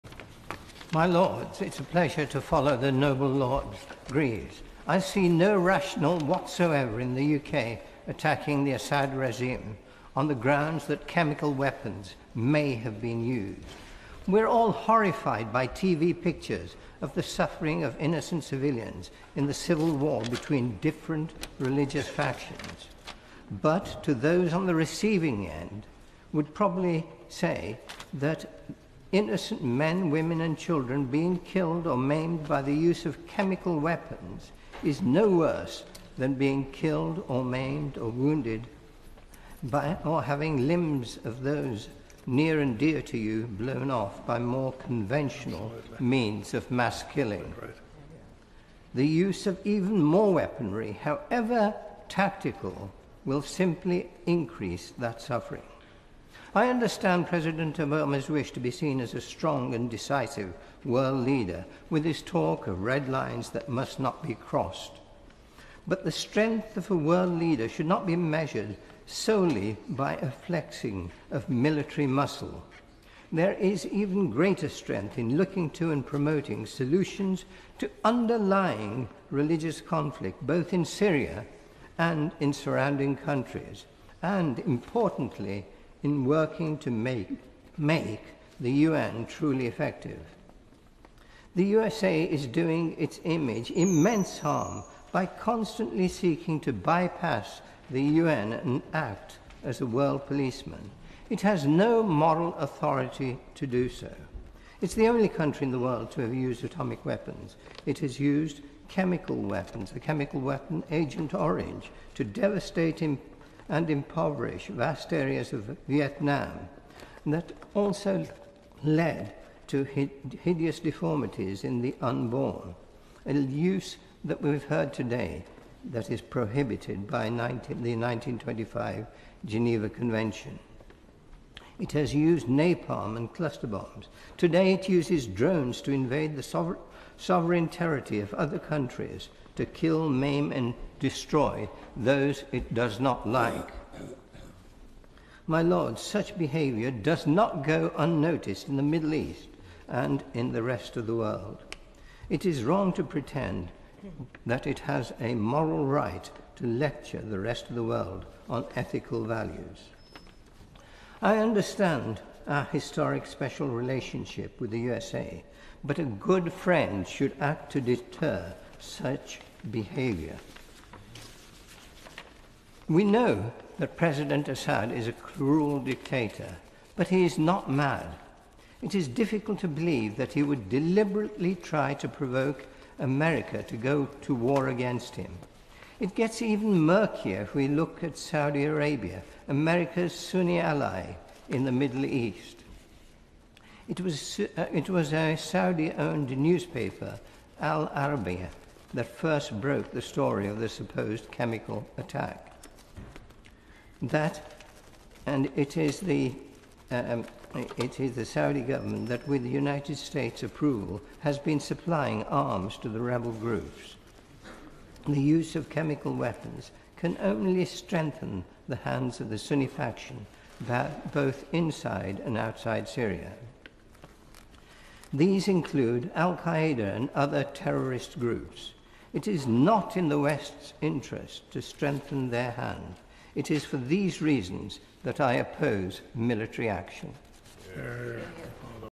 Syria and the Use of Chemical Weapons l 29 August 2013
Lord Singh of Wimbledon's contribution on the Syria and the Use of Chemical Weapons debate in the House of Lords